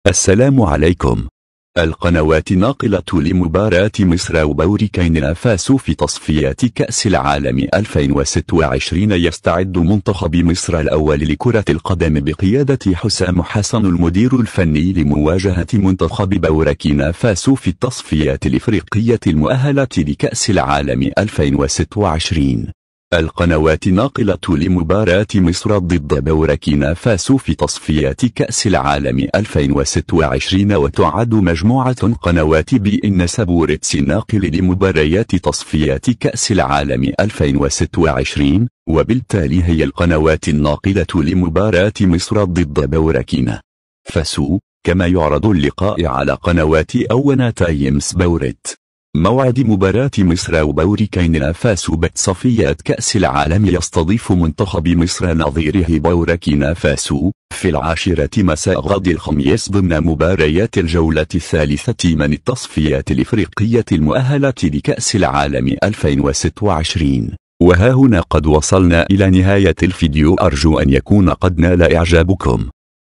القنوات الناقلة لمباراة مصر وبوركينا فاسو في تصفيات كأس العالم 2026
يستعد منتخب مصر الأول لكرة القدم بقيادة حسام حسن المدير الفني لمواجهة منتخب بوركينا فاسو في التصفيات الإفريقية المؤهلة لكأس العالم 2026.
القنوات الناقلة لمباراة مصر ضد بوركينا فاسو في تصفيات كأس العالم 2026
وتُعَد مجموعة قنوات بي إن سبورتس الناقل لمباريات تصفيات كأس العالم 2026، وبالتالي هي القنوات الناقلة لمباراة مصر ضد بوركينا فاسو، كما يعرض اللقاء على قنوات أون تايم سبورت.

موعد مباراة مصر وبوركينا فاسو بتصفيات كأس العالم
 يستضيف منتخب مصر نظيره بوركينا فاسو، في العاشرة مساء غد الخميس ضمن مباريات الجولة الثالثة من التصفيات الإفريقية المؤهلة لكأس العالم ٢٠٢٦. Follow us on AMR_VIP